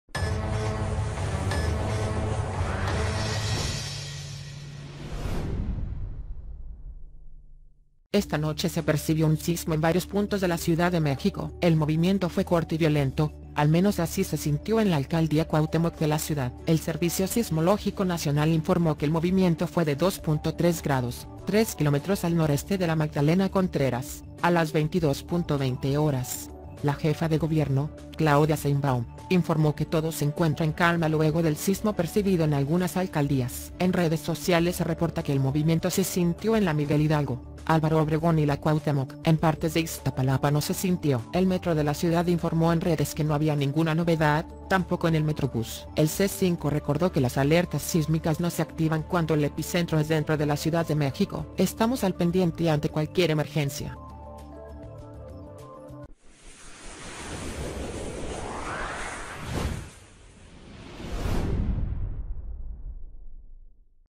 Sismo de 2.3 con epicentro en la Magdalena Contreras estremece a la CDMX
Esta noche se percibió un sismo en varios puntos de la Ciudad de México. El movimiento fue corto y violento, al menos así se sintió en la alcaldía Cuauhtémoc de la ciudad.

El Servicio Sismológico Nacional informó que el movimiento fue de 2.3 grados, tres kilómetros al noreste de la Magdalena Contreras, a las 22.20 horas.

La Jefa de Gobierno, Claudia Sheinbaum, informó que todo "se encuentra en calma luego del sismo percibido en algunas alcaldías".

El Metro de la Ciudad informó en redes que no había ninguna novedad, tampoco en el Metrobús.

El C5 recordó que "las alertas sísmicas no se activan cuando el epicentro es dentro de la Ciudad de México. Estamos al pendiente ante cualquier emergencia".